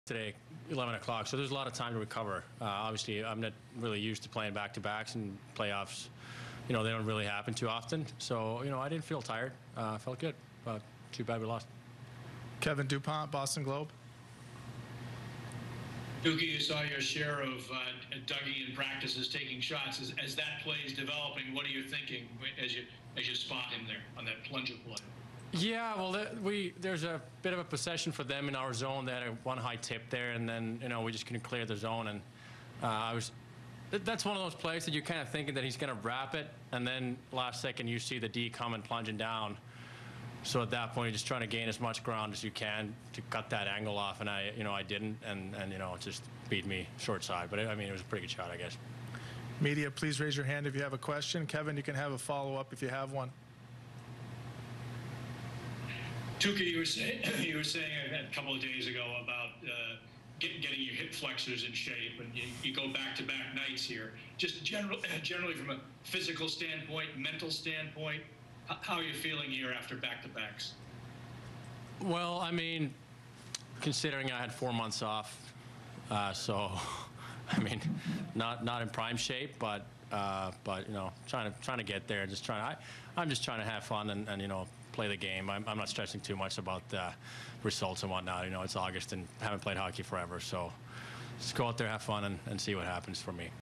Bruins Goalie Tuukka Rask Reacts To Game 2 Loss To Hurricanes
"I didn't feel tired, I felt good. It's too bad we lost."

Bruins netminder Tuukka Rask reacts to the squad's Game 2 loss to the Hurricanes.